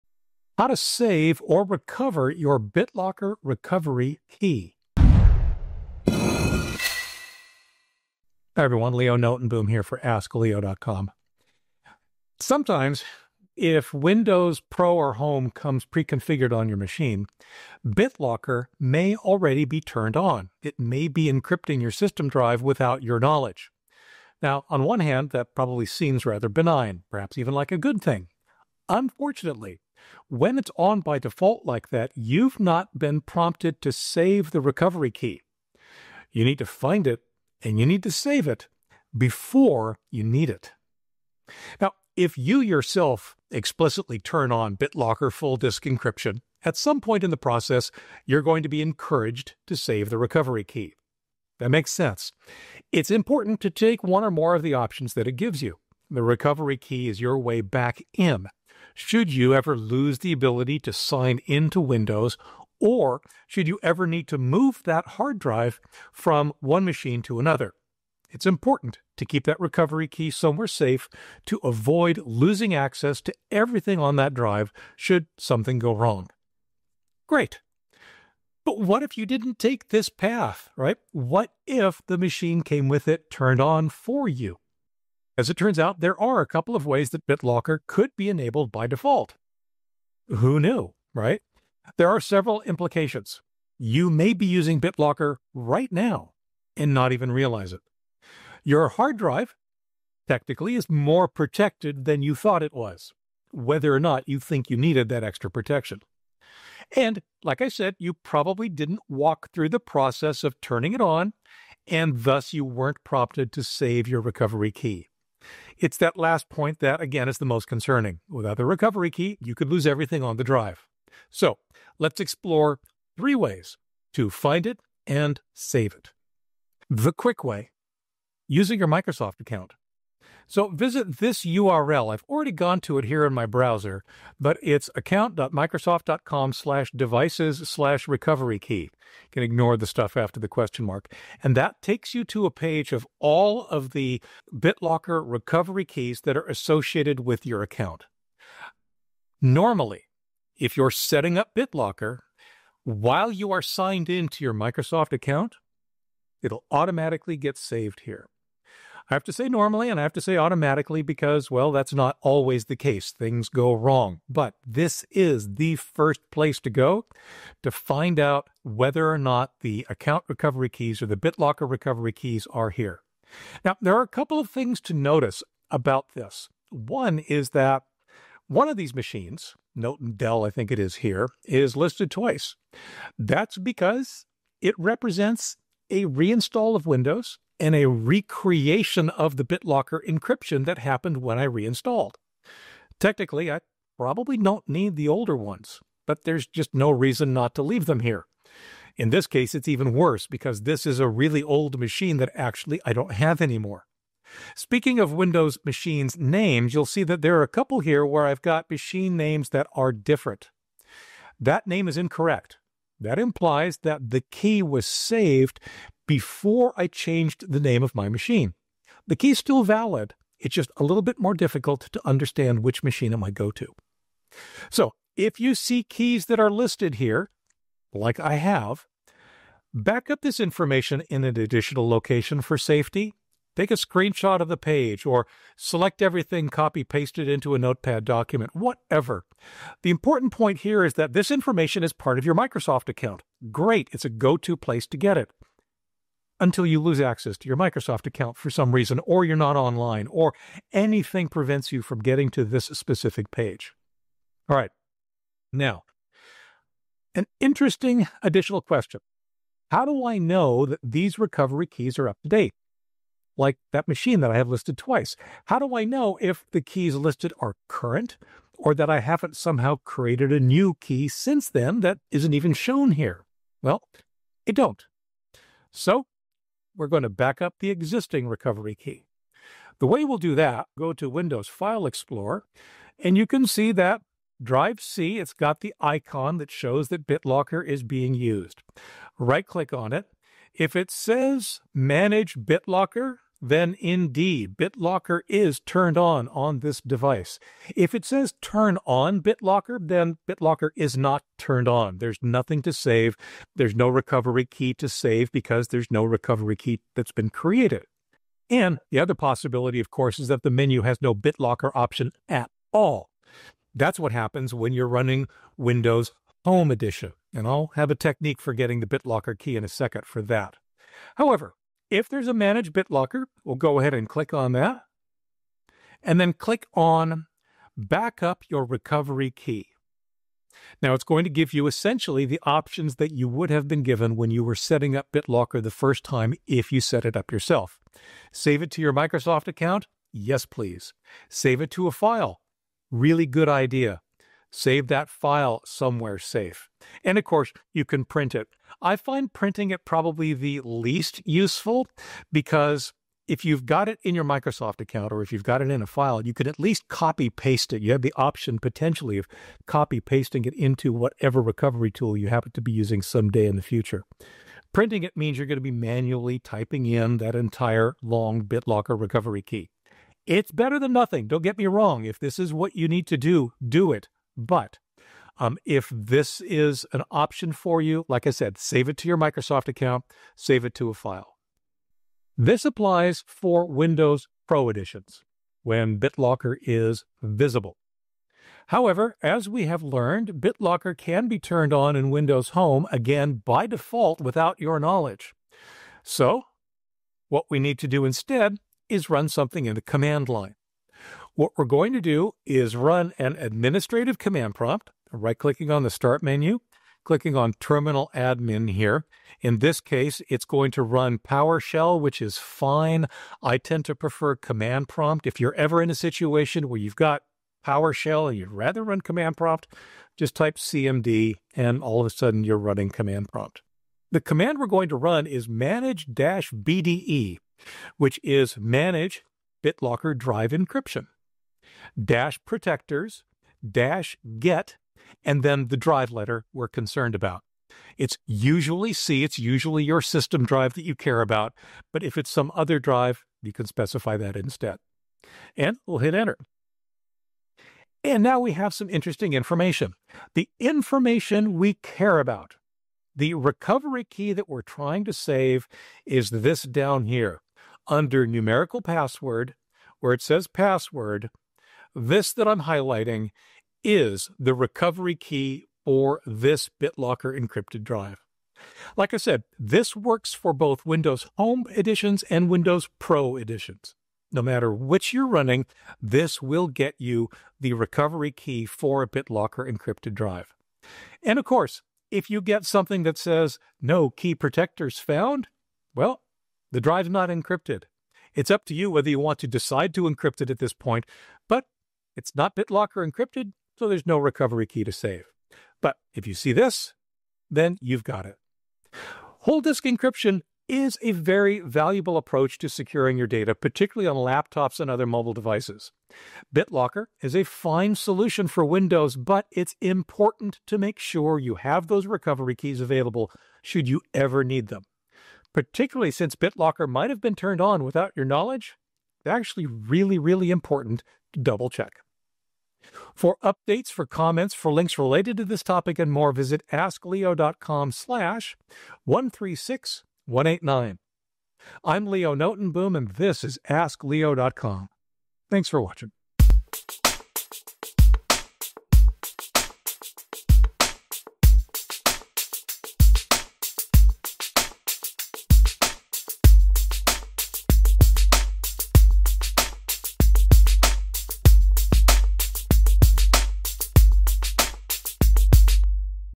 How to Save or Recover Your BitLocker Recovery Key
❌ BitLocker could be running on your PC without your knowledge. Without the recovery key, you risk losing everything. I'll show you three ways to check, find, and save your key.

❌ Finding your BitLocker recovery key
You can find your BitLocker Recovery key:
  - When you enable BitLocker yourself.
  - In your Microsoft account online.
  - By copying it from the “Manage BitLocker” interface.
  - Via the “manage-bde” command-line tool.
Regardless of how you get it, save it somewhere safe.

Chapters
0:00 Save or Recover Your BitLocker Recovery Key
0:40 Traditional BitLocker setup
1:20 Bitlocker on by default
2:00 Three ways to find it and save it
2:07 Use your Microsoft account
4:35 Windows File Explorer: back up your recovery key
6:45 The Command Prompt: Windows Home or Pro